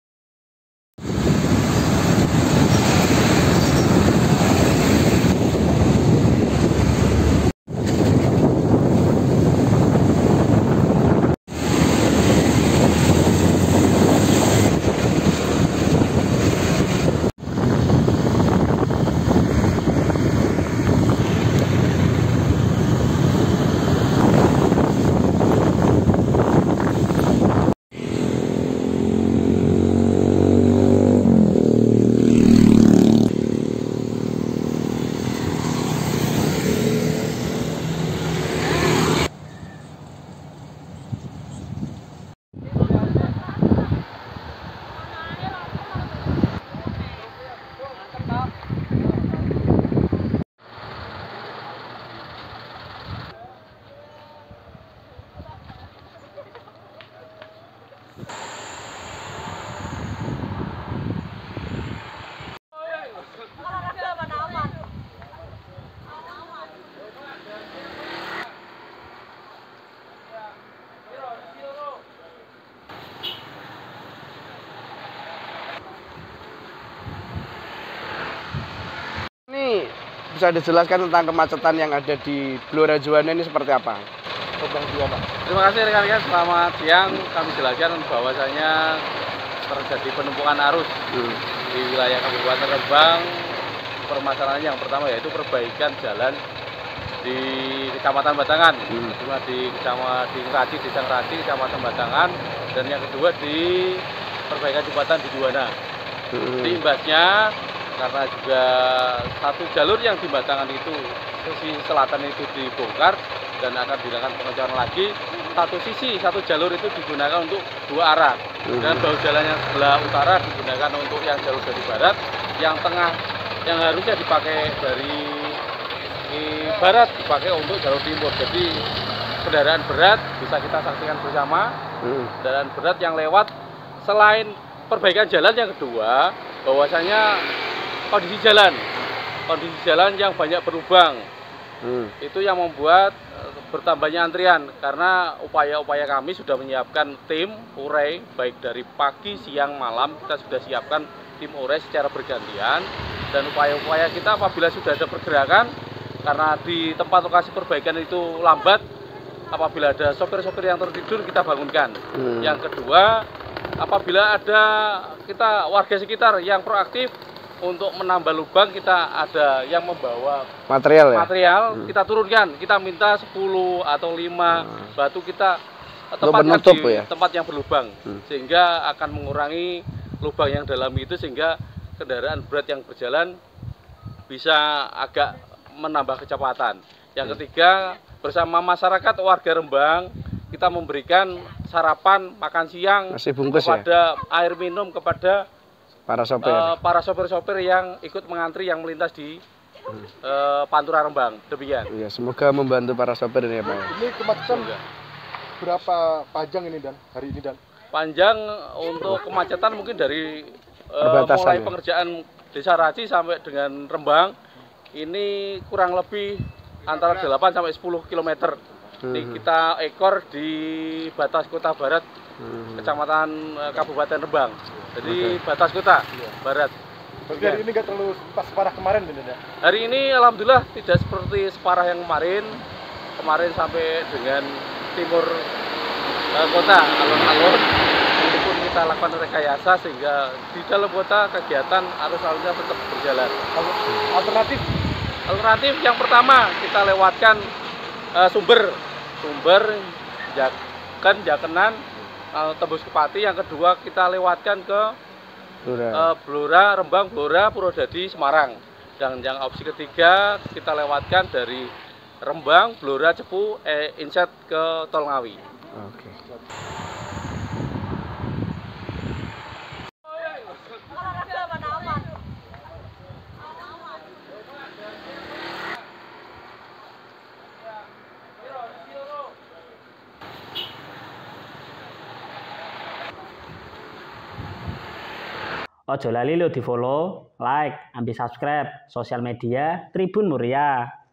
Imbas Terjebak Macet, Petugas Satlantas Polres Rembang Berikan Makanan Kepada Para Sopir
Lantaran macet panjang  di jalan Pantura, Petugas Satlantas Polres Rembang melakukan bagi-bagi nasi bungkus kepada para sopir di jalan pantura Rembang.

Menurut informasi hingga saat ini kemacetan masih mengular hingga 10 Km di jalur Pantura Pati-Rembang.

Karena antrean yang cukup panjang, Satlantas Rembang membagikan air mineral maupun makanan untuk para sopir yang terjebak kemacetan.

Selain perbaikan jalan, kondisi jalan yang banyak berlubang yang membuat bertambahnya antrean.